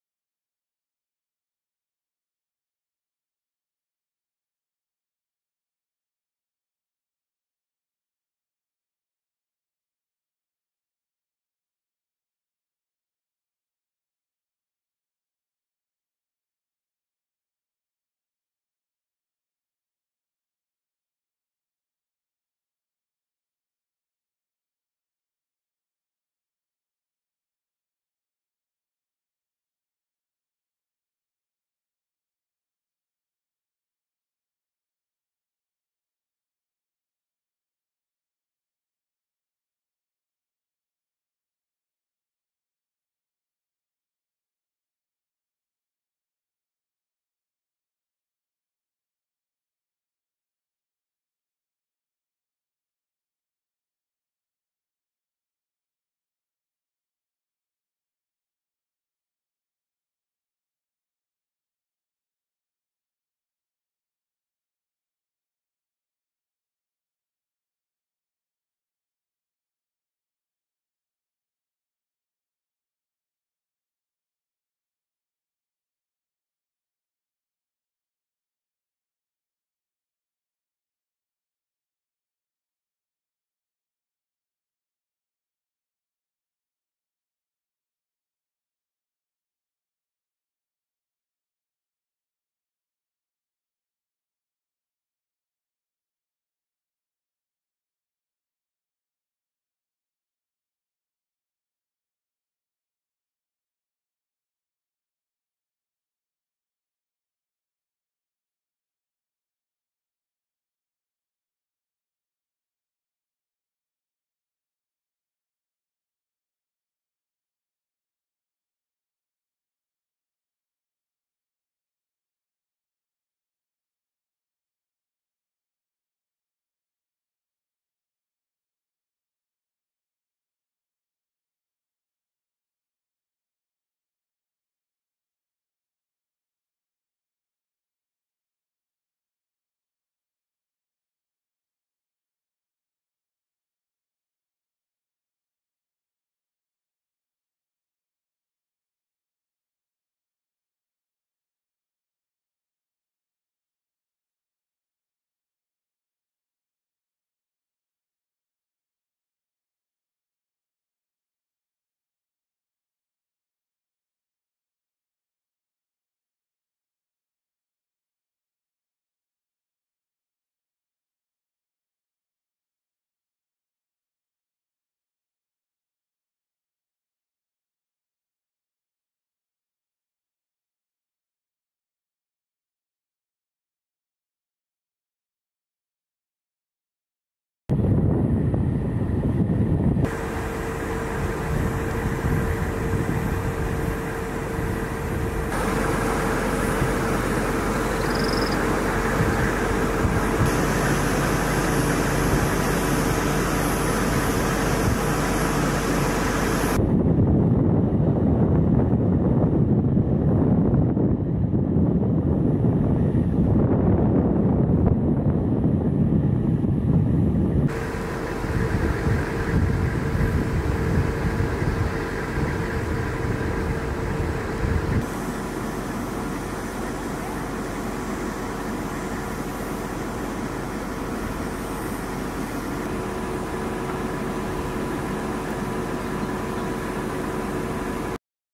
WATCH: Texas Employs New Buoy Barriers In Rio Grande River, Razor Wire To Stop Illegal Migration
Texas has installed new buoy barriers in the Rio Grande River along with razor wire to deter illegal migration.